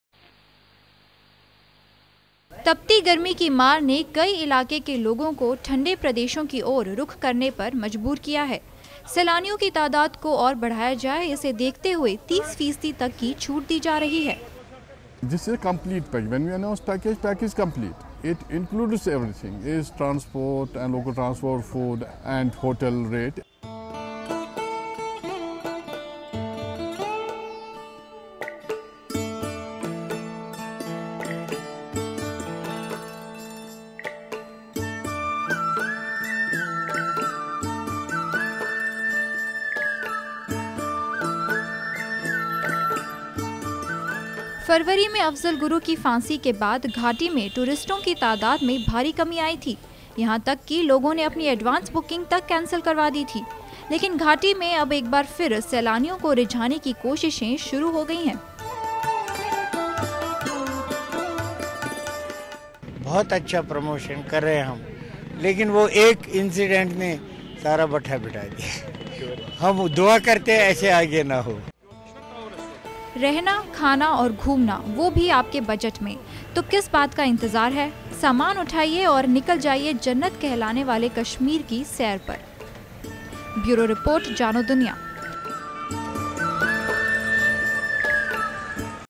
J&K Government To Organise Chain Of Activities In Tourist Season - JanoDuniya
Jammu and Kashmir government is contemplating to organize a chain of activities during the current tourism season, as part of its efforts to attract large number of tourists and showcase the rich cultural heritage and major products of the state.Read more